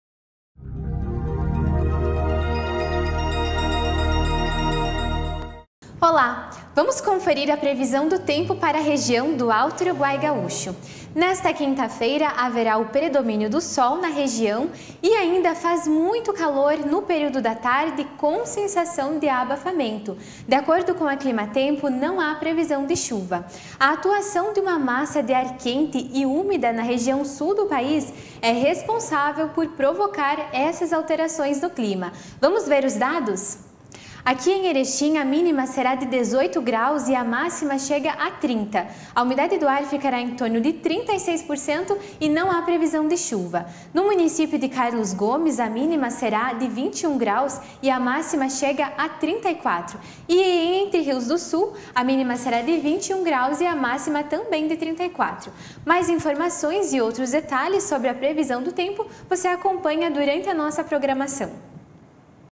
Previsão do Tempo: 16/12/2021
TV BOM DIA

Quinta-feira de sol, com algumas nuvens